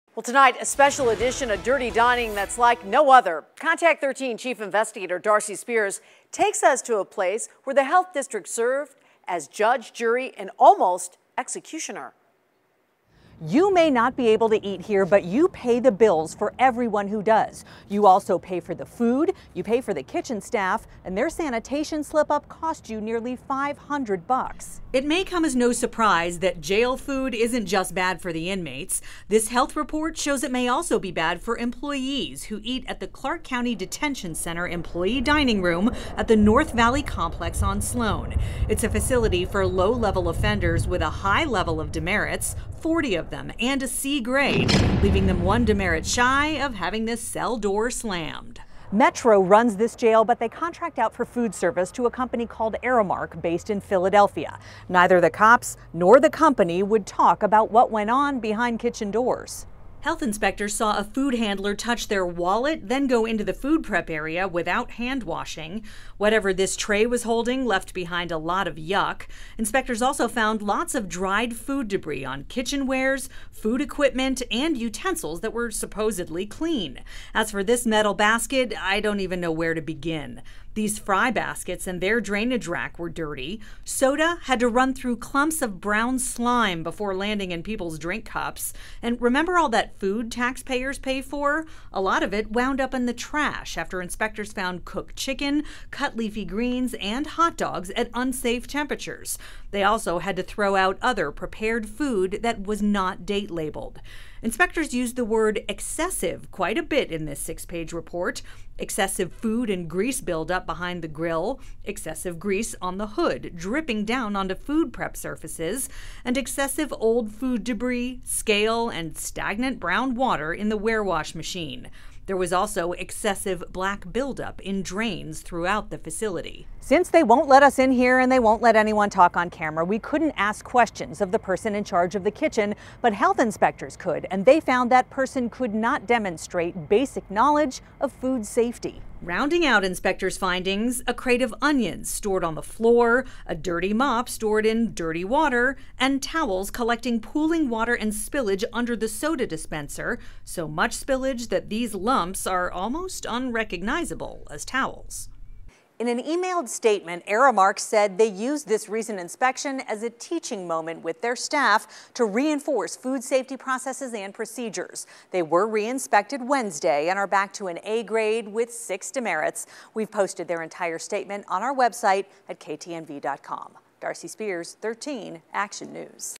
Clark County Detention Center employee dining room on Dirty Dining
The health report shows it may also be bad for employees who eat at the Clark County Detention Center employee dining room at the North Valley Complex on Sloan Lane and Las Vegas Boulevard.